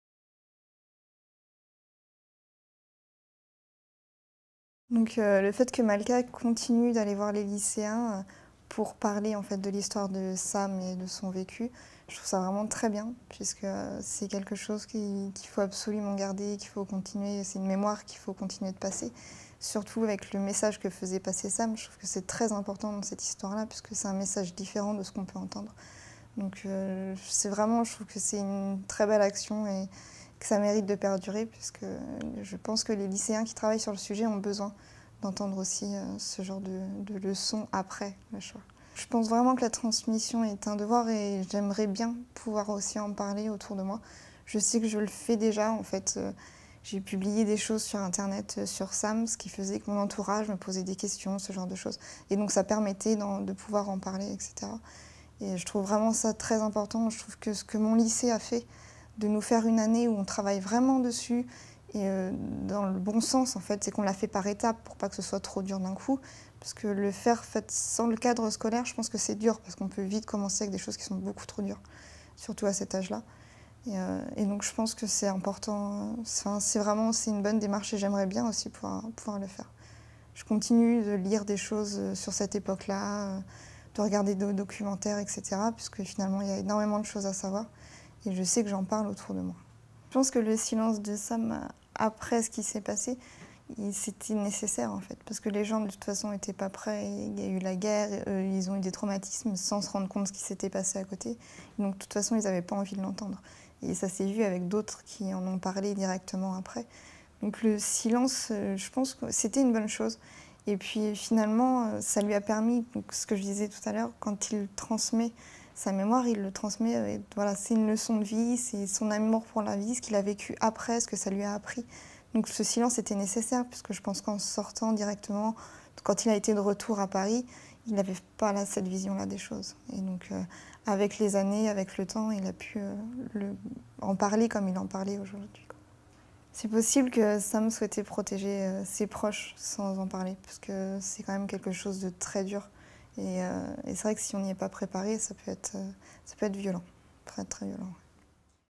Laurie Dupuis - La Mémoire
Laurie Dupuis nous parle de l'importance de la Mémoire.